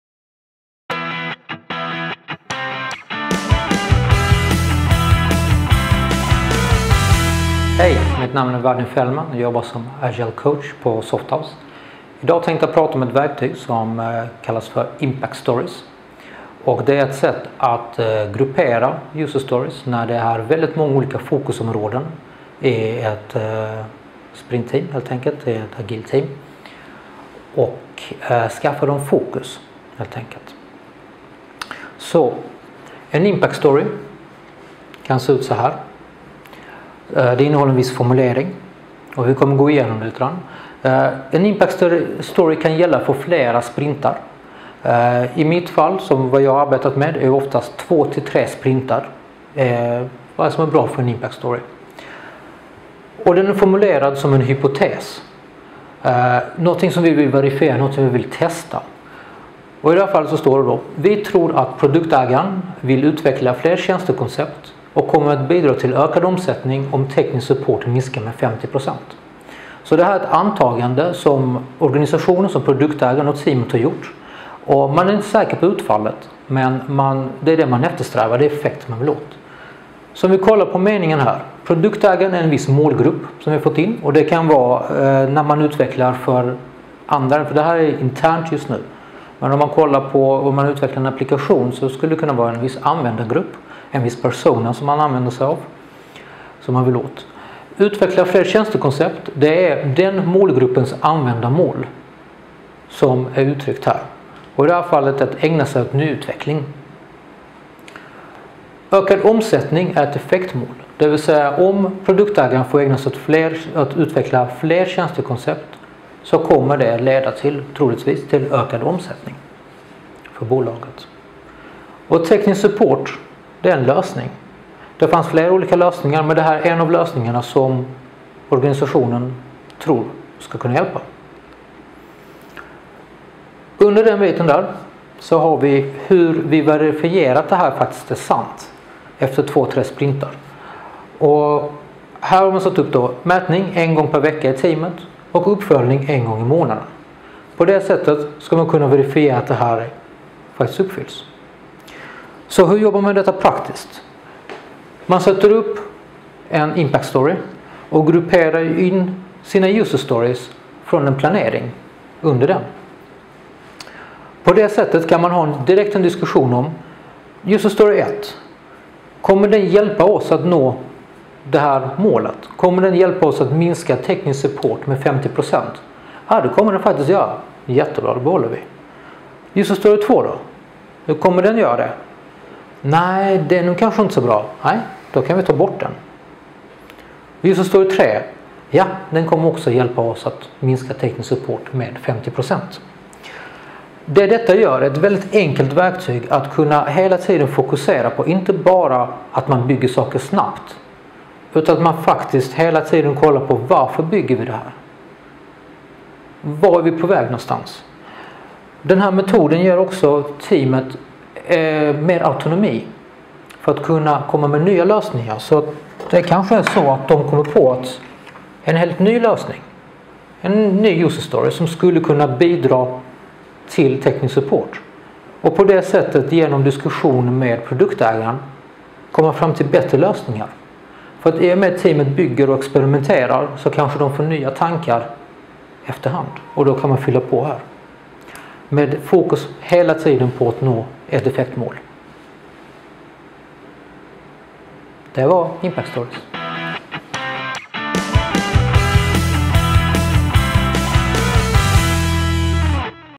Impact Stories - white board video
Vadim Feldman ger oss en introduktion till Impact Stories.

Impact Stories är ett sätt att gruppera user stories när ett agilt team har väldigt många fokusområden.